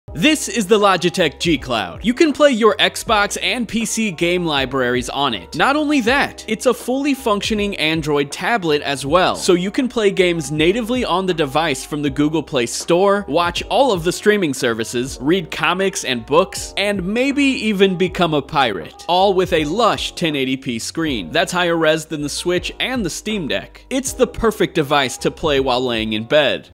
The FUTURE of Handheld Gaming?
Is the Logitech G Cloud worth it? I think so! The G Cloud is a fully functional Android Tablet. Meaning it’s a multimedia device capable of watching movies, playing games natively, reading comics, emulation, and more! However, it’s also perfect for streaming Xcloud and Nvidia GeForce Now. Here’s my Logitech G Cloud review!

G Cloud provided by Logitech for review purposes